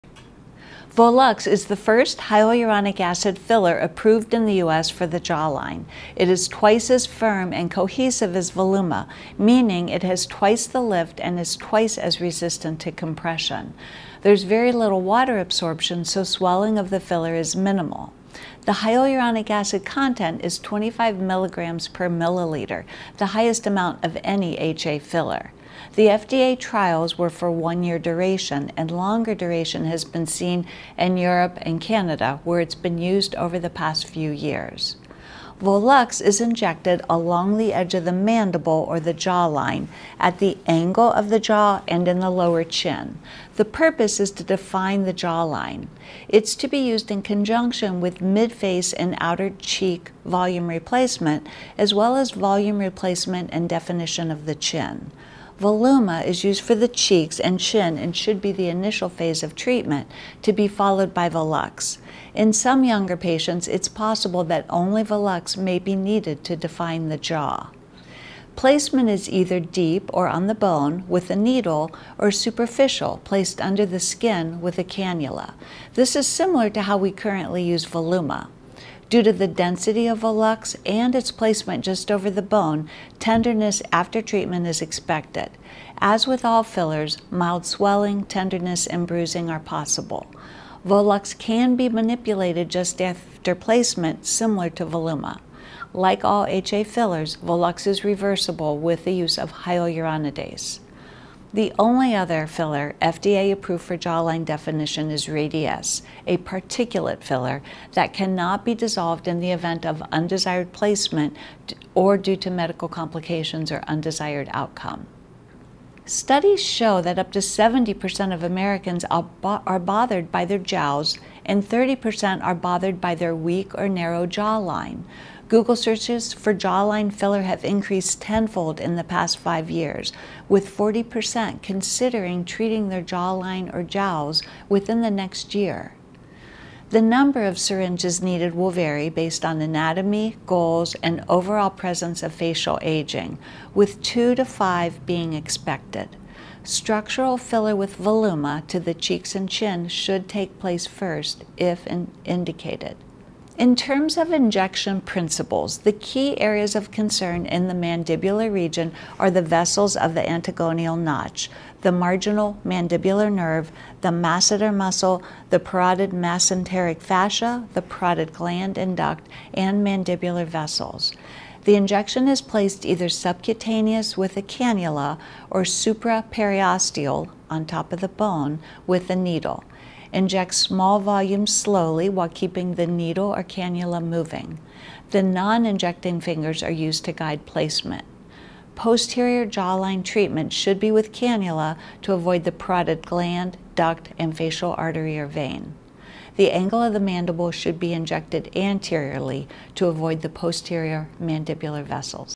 Best Facial Fillers and Their Differences - Juvederm Volux vs. Voluma
Karen Whitney, PA with Advanced Cosmetic Surgery & Laser Center discusses the differences between Juvederm Voluma and Juvederm Volux for facial rejuvenation.

When it comes to facial fillers, there are a lot of options out there, and it can be difficult to know which one is right for you. Two popular options are Volux and Voluma, both made by the company Juvederm. While both of these fillers build volume and create lift, they have some important differences.

One of the main differences between Volux and Voluma is where they are injected. Voluma is FDA-approved for the chin and cheeks, while Volux is FDA-approved to treat the chin and jawline. This means that if you're looking to sculpt and reshape your chin and jawline, Volux may be the better choice for you.

Another difference is the consistency of the fillers. Volux has more structure, cohesivity, and lift capacity than Voluma, making it a lot firmer. This firmness is what allows Volux to provide that chiseled jawline that many people are seeking. In fact, Volux is a slightly more cohesive and thicker formula than Voluma, which makes it mimic bone.

Volux  is injected along the edge of the mandible or the jawline, at the angle of the jaw and in the lower chin. The purpose is to define the jawline. It's to be used in conjunction with mid-face and outer cheek volume replacement as well as volume replacement and definition of the chin. Voluma is used for the cheeks and chin and should be the initial phase of treatment to be followed by Volux . In some younger patients, it's possible that only Volux  may be needed to define the jaw placement is either deep or on the bone with a needle or superficial placed under the skin with a cannula.

This is similar to how we currently use Voluma. Due to the density of Volux  and its placement just over the bone. Tenderness after treatment is expected. As with all fillers, mild swelling, tenderness and bruising are possible. Volux  can be manipulated just after placement similar to Voluma. Like all fillers, Volux  is reversible with the use of Hyaluronidase. The only other filler FDA approved for jawline definition is Radiesse, a particulate filler that cannot be dissolved in the event of undesired placement or due to medical complications or undesired outcome.

Conclusion
In conclusion, Volux facial filler is a cutting-edge solution for individuals looking to combat the signs of aging. With its long-lasting results, natural-looking outcome, and minimal downtime, it's no wonder that this product is rapidly gaining popularity among those seeking to rejuvenate their appearance. If you're looking to restore a more youthful and refreshed look, Volux may be the right solution for you

Volux is the first hyaluronic acid filler approved in the US for the jawline. It is twice as firm and cohesive as Voluma, meaning it has twice the lift and is twice as resistant to compression. There's very little water absorption, so swelling of the filler is minimal. The hyaluronic acid content is 25mg/ml, the highest amount of any HA filler. The FDA trials were for one year duration and longer duration has been seen in Europe and Canada, where it's been used over the past few years.

Vimeo Title:  Volux vs Voluma Final